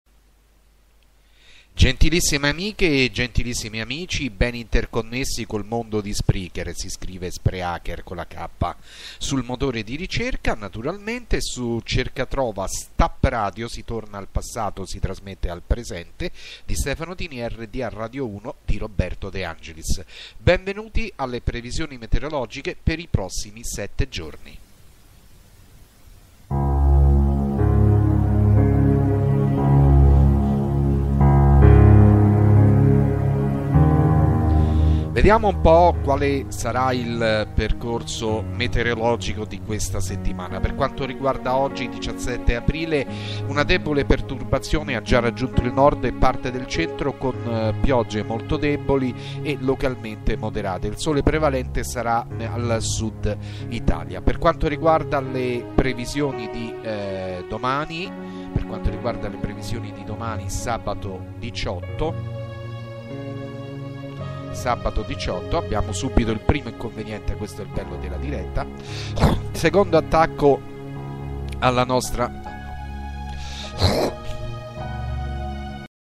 METEO stap 17/4-23/4
previsioni meteo 17/4-23/4 - in studio roberto de angeliss in regia stefano tini